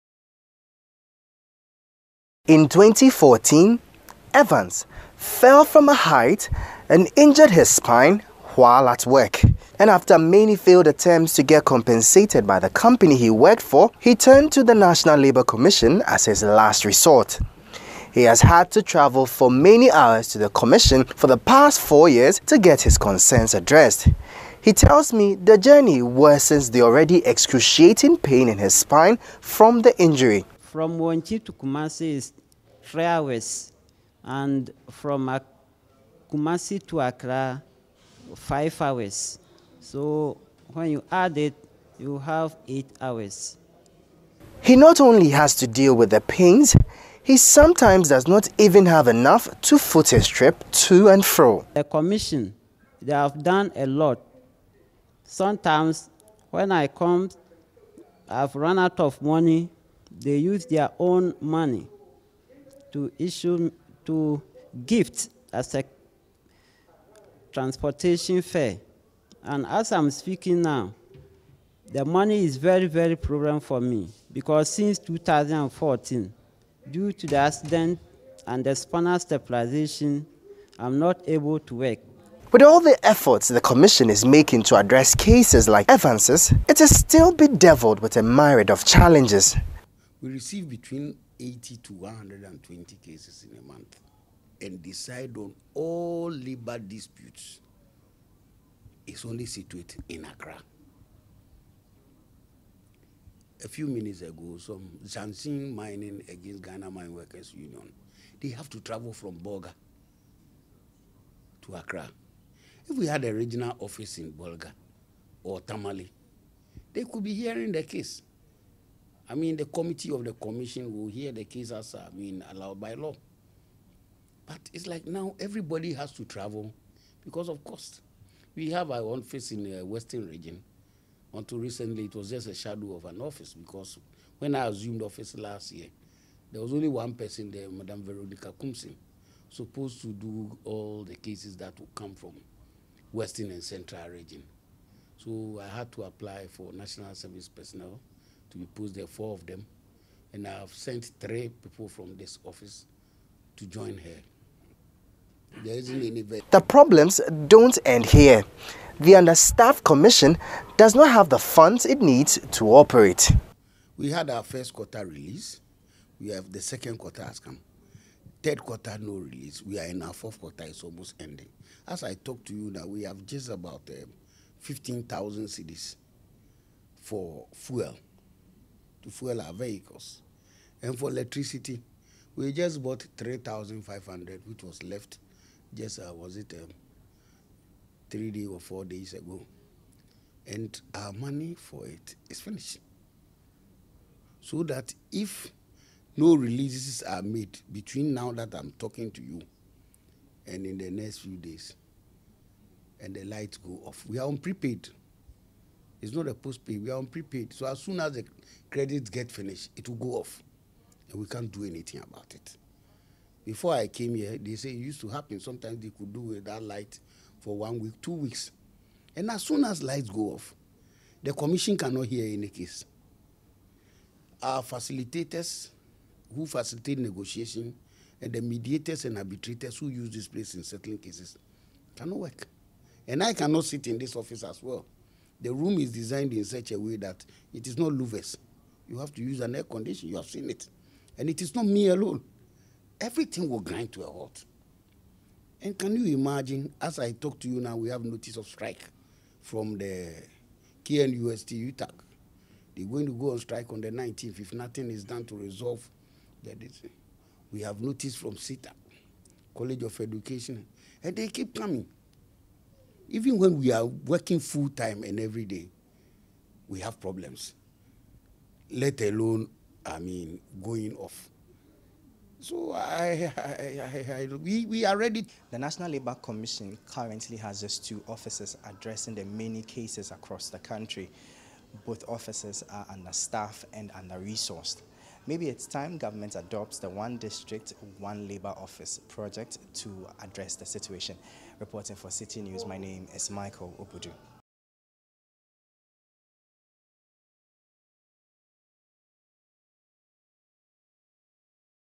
NATIONAL LABOUR COMMISSION CASH TRAPPED
The National labour Commission may have to halt operations soon because it is broke. According to the Commission, government has failed to release funds to the commission for several months.
The National Labour Commission (NLC) was established under Section 135 of the Labour Act, 2003 (Act 651) and it functions include the settlement of industrial dispute.
There is more in the following report